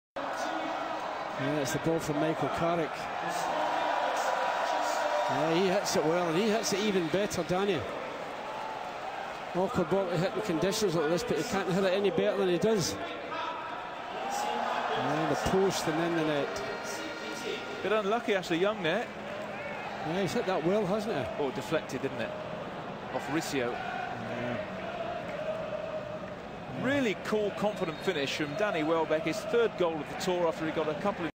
[Tour 2013] Kitchee FC 0-1 Manchester United (Danny Welbeck)
Tour 2013: Kitchee FC vs Manchester United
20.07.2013